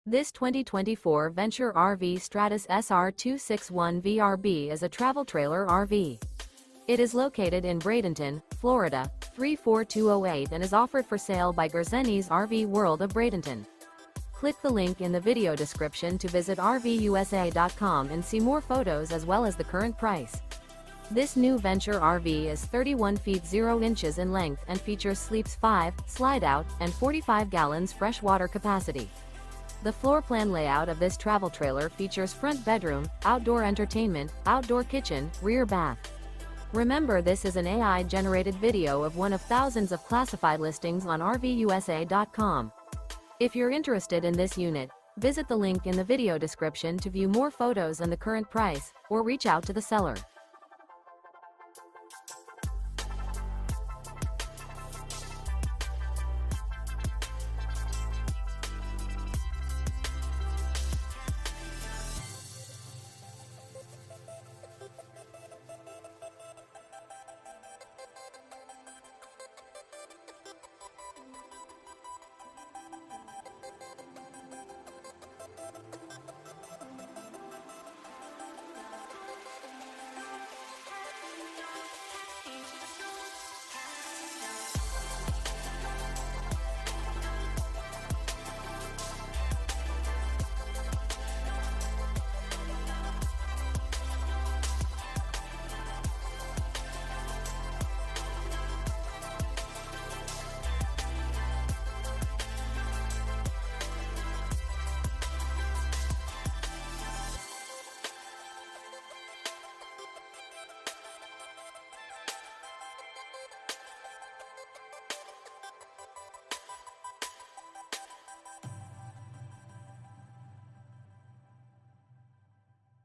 Magnificent 2024 Venture RV Stratus Travel Trailer RV For Sale in Bradenton, FL | RVUSA.com
This 2024 Venture RV Stratus SR261VRB is a Travel Trailer RV. It is located in Bradenton, FL 34208 and is offered for sale by Gerzeny's RV World of Bradenton. This New Venture RV Is 31' 0" in length and features sleeps 5, Slideout, and 45 gal fresh water capacity. The floorplan layout of this Travel Trailer features Front Bedroom, Outdoor Entertainment, Outdoor Kitchen, Rear Bath.